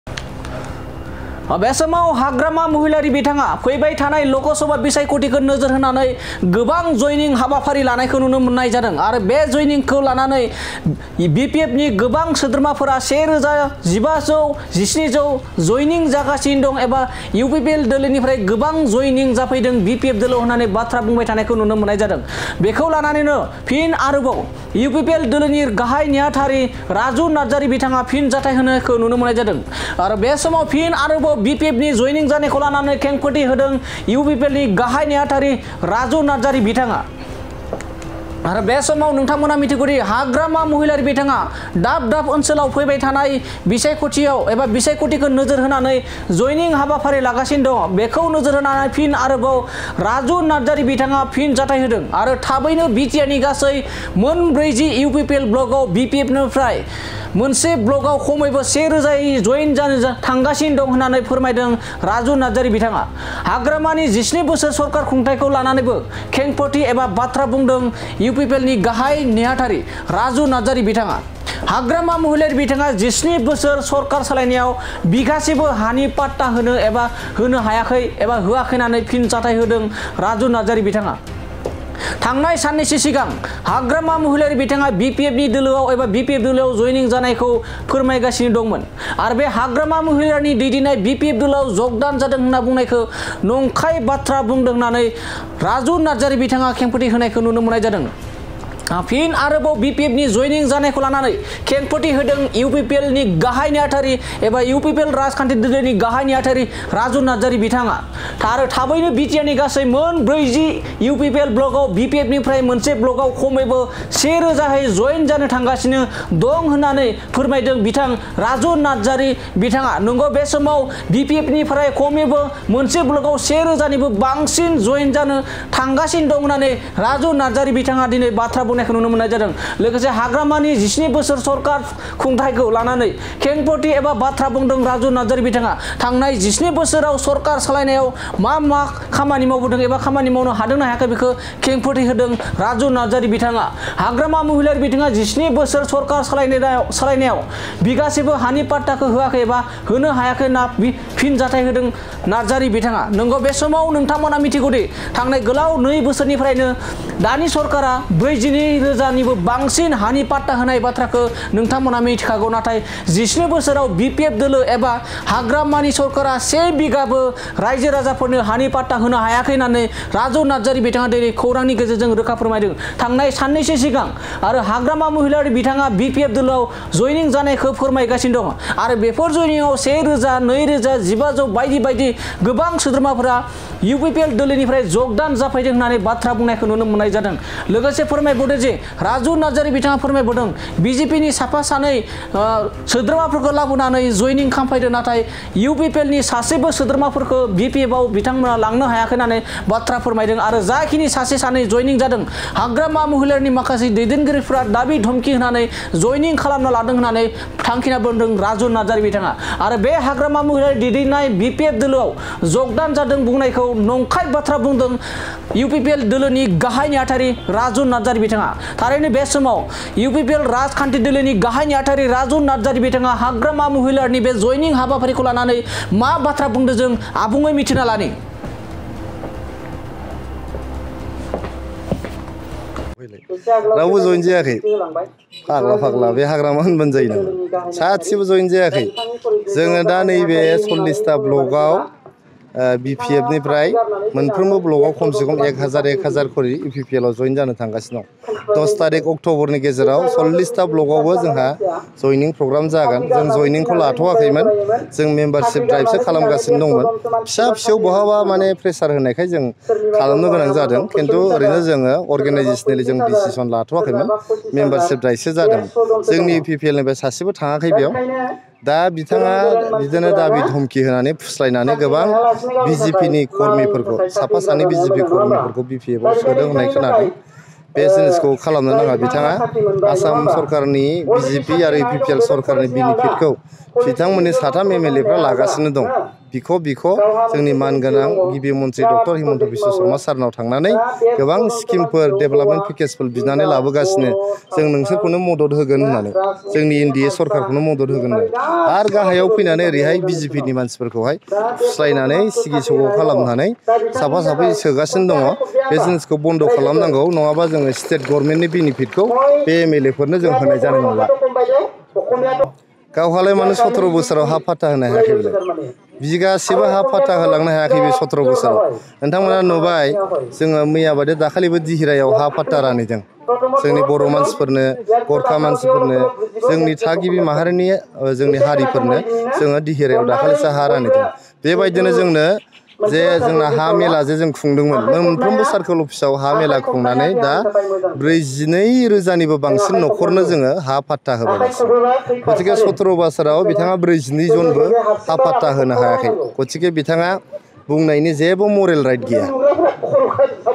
मोनसे ब्लकआव खमैबो से रोजाहै जयन जानो थांगासिनो दं होनना फोरमायदों बिथां राजु नार्जारिया।
BTRनि गासै मोन 40 UPPL ब्लकआव BPF निफ्राय मोनसे ब्लकआव खमैबो से रोजाहै जयन जानो थांगासिनो दं होनना फोरमायदों बिथां राजु नार्जारिया।